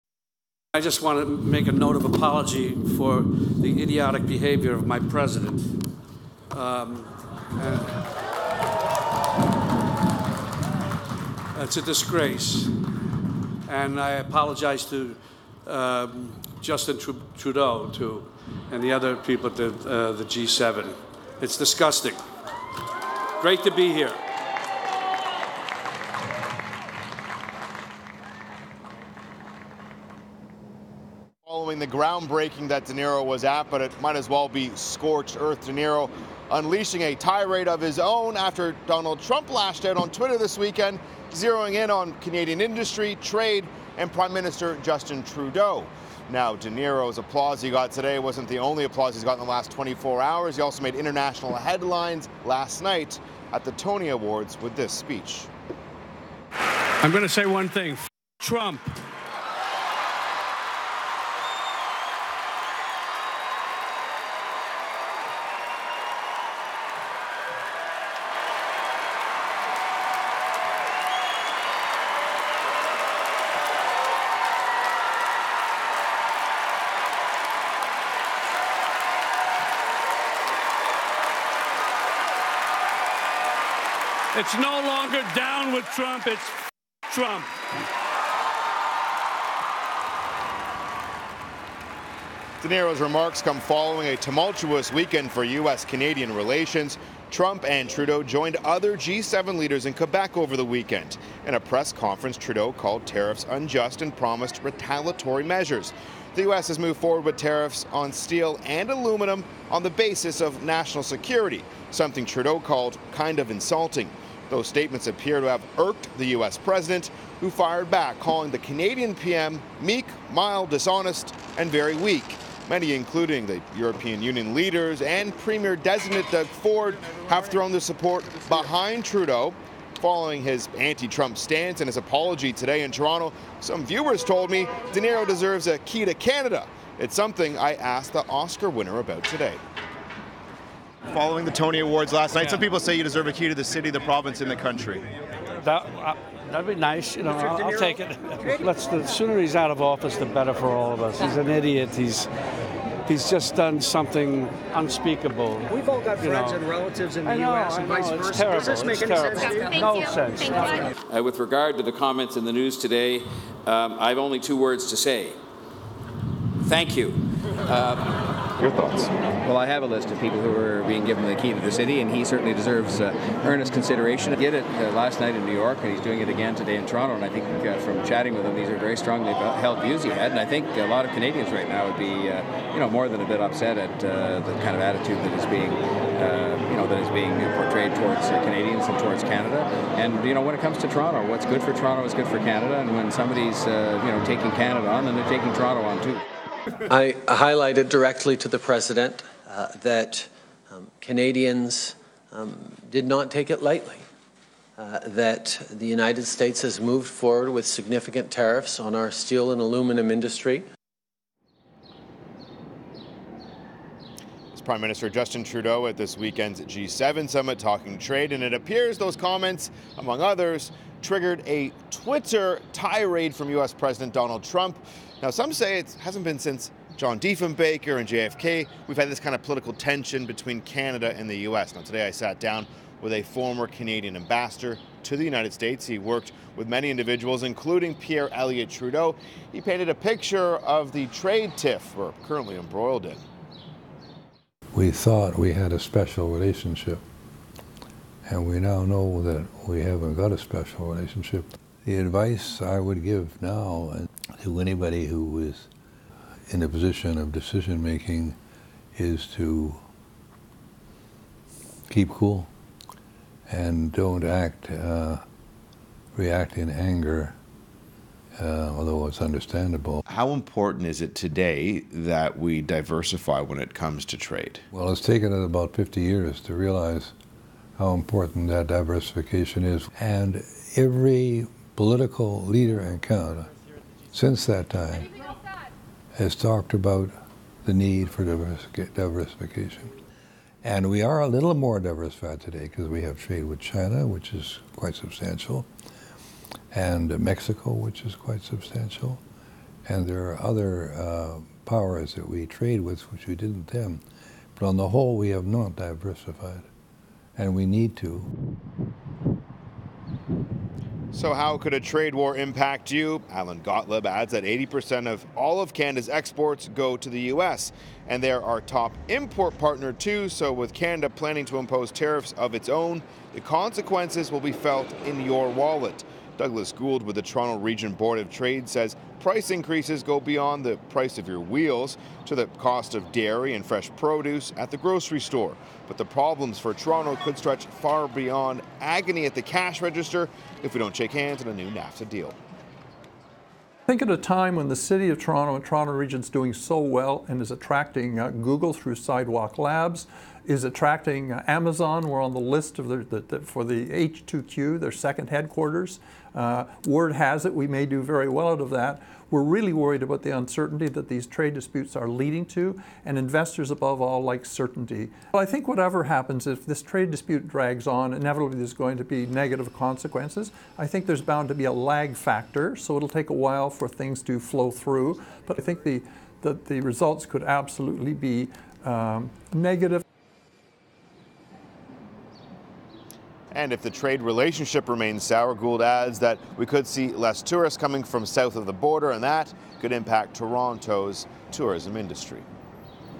Robert De Niro lashes out at Donald Trump while speaking in Toronto
Actor Robert De Niro, while speaking in Toronto Monday, apologized to Canadians for Donald Trump's comments about Justin Trudeau. Adrian Ghobrial with the latest in the tough talk over tariffs and trade.